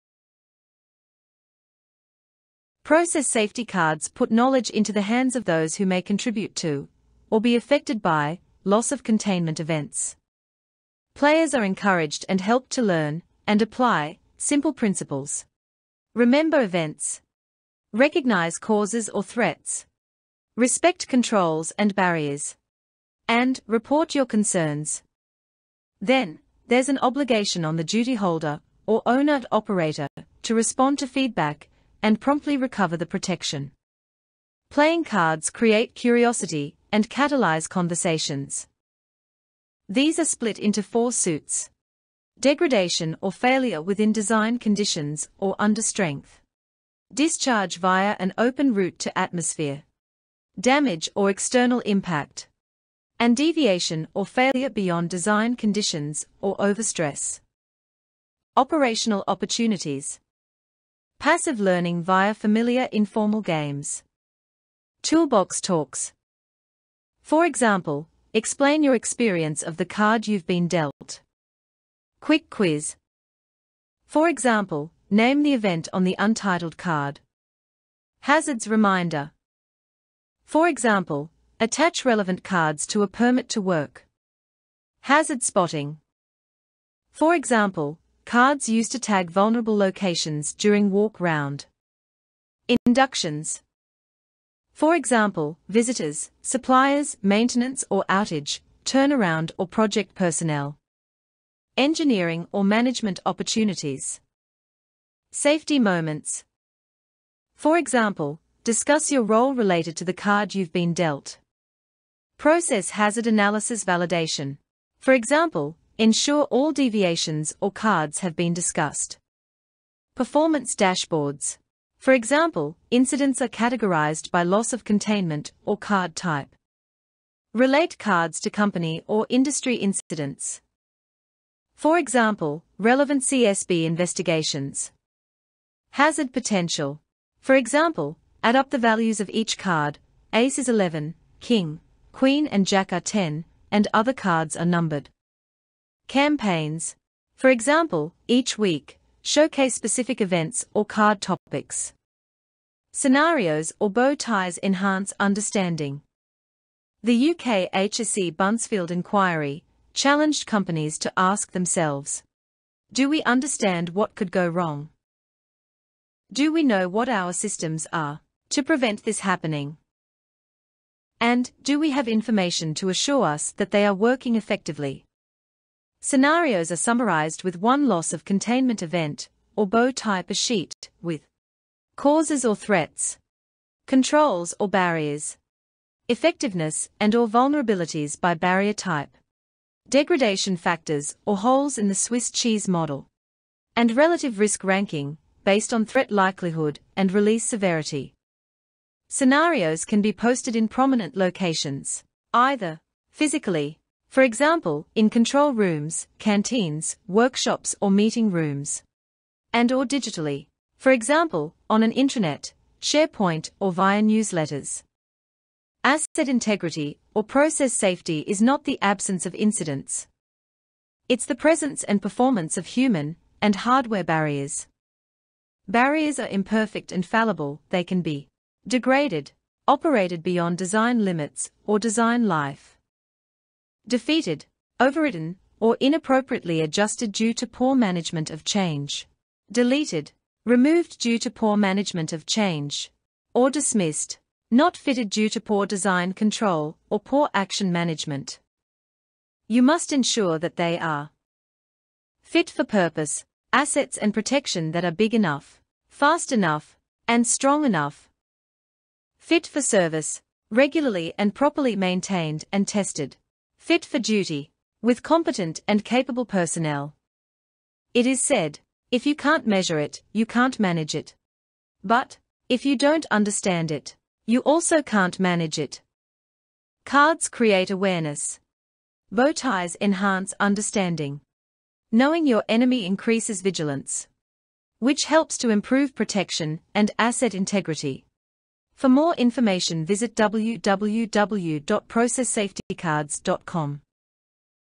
Introduction to Process Safety Cards
A brief (5 min) introduction to the concept of Process Safety Cards and how they can be used to engage a wide range of stakeholders by informing novices & reminding veterans of the potential for & avoidance of Loss of Containment (LoC) events associated with hazardous materials or energy.